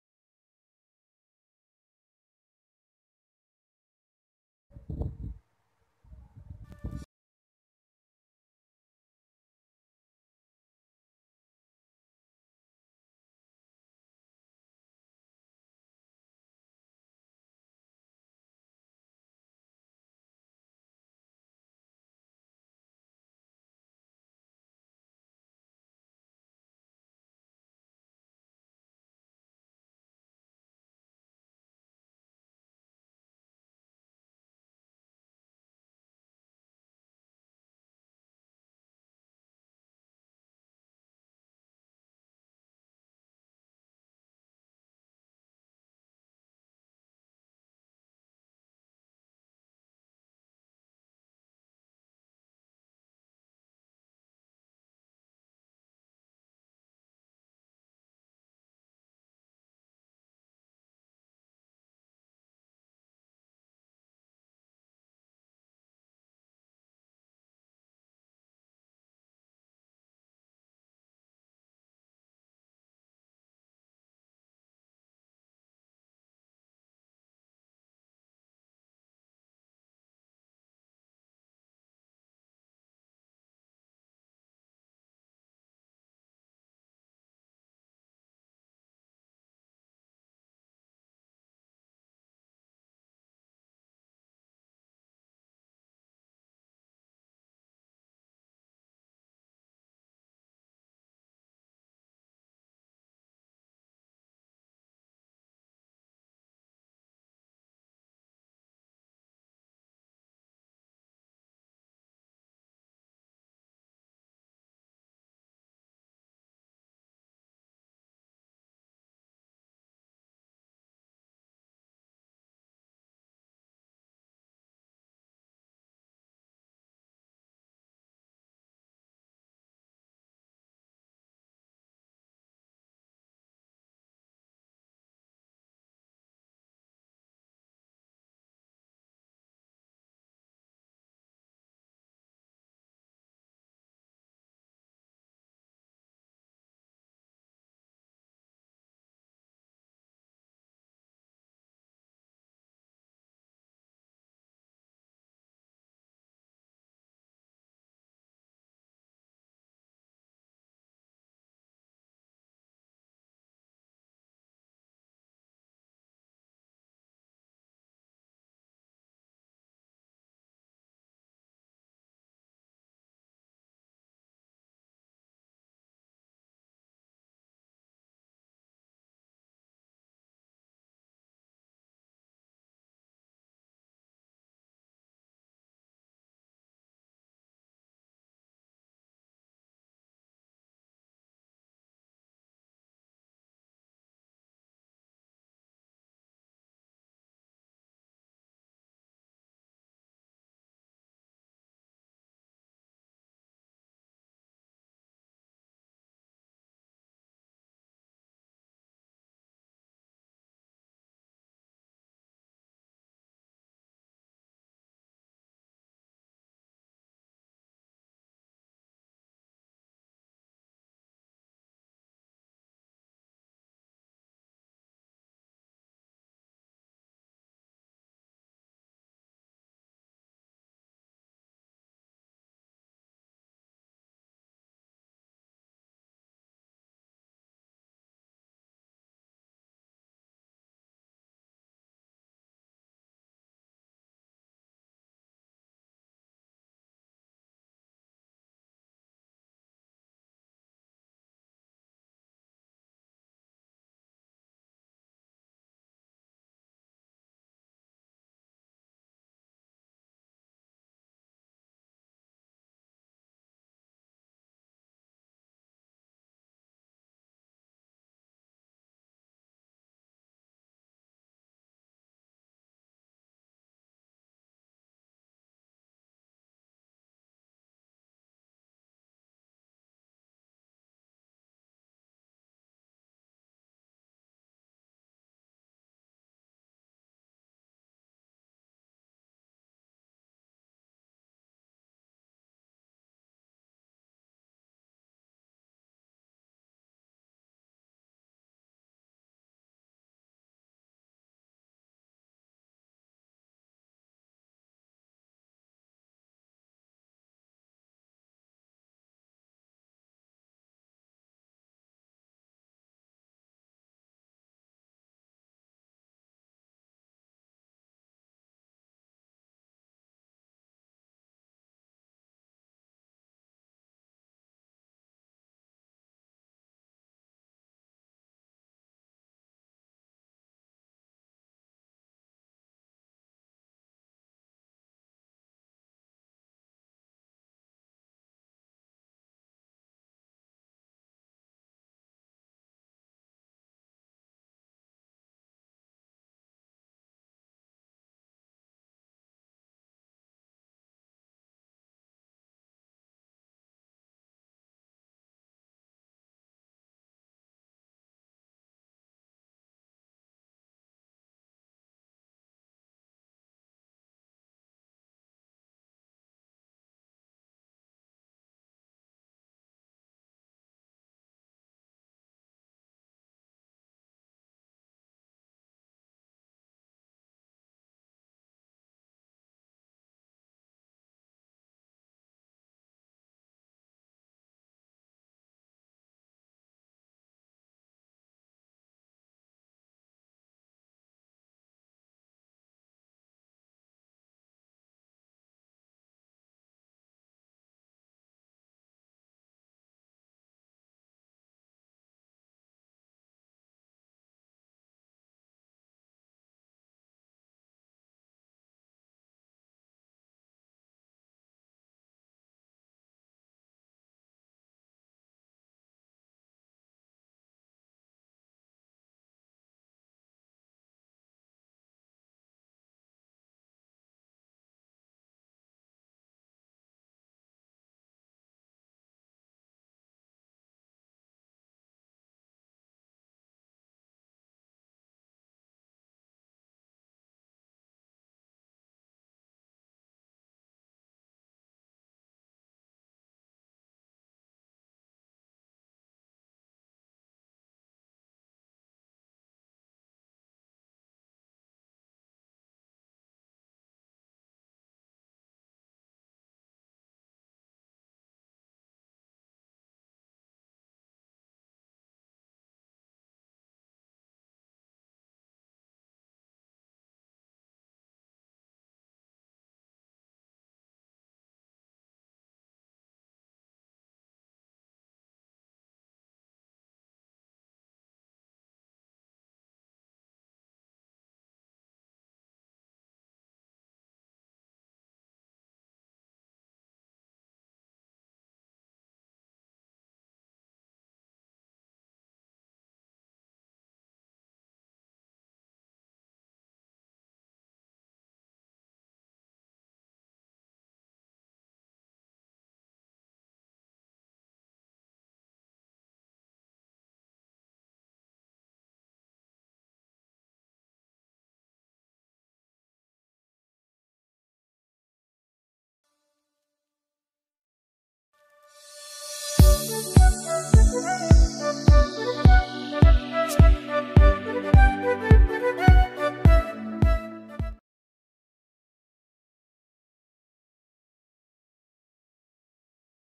DIY Unique Woolen Basket out of waste//Home Decor Idea//Newspaper Craft//Handmade Craft.
Hello Friends
      It is very easy to make.This beautiful basket made with newspaper and woolen.

Plastic